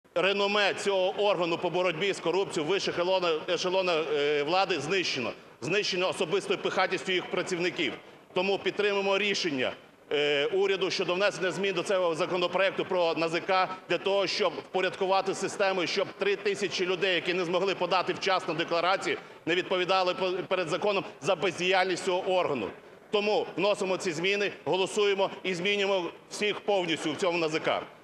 "Народний фронт" підтримує урядову ініціативу змін до закону про НАЗК, - Бурбак
Реноме Національного агентства з питань запобігання корупції повністю знищено завдяки діям його працівників.

Таку думку висловив керівник фракції "Народний фронт" Максим Бурбак, виступаючи в парламенті.

"Реноме цього органу по боротьбі з корупцією у вищих ешелонах влади знищено  особистою пихатістю його працівників", - заявив Бурбак.

Він наголосив на необхідності прийняття змін до закону про НАЗК, яким забезпечити продовження терміну подання електронних декларацій для держслужбовців, які не змогли подати вчасно декларації, та зміни, які дозволять оновити неефективний кадровий склад агентства.

"Ми підтримуємо рішення уряду щодо внесення змін до законопроекту про НАЗК для того, щоб впорядкувати систему і щоб понад 3 тисячі державних службовців, які не змогли подати вчасно декларацію, не відповідали перед законом за бездіяльність цього органу", - підкреслив Максим Бурбак.

"Тому голосуємо і змінюємо усіх в НАЗК", - резюмував він.

Приєднуйся!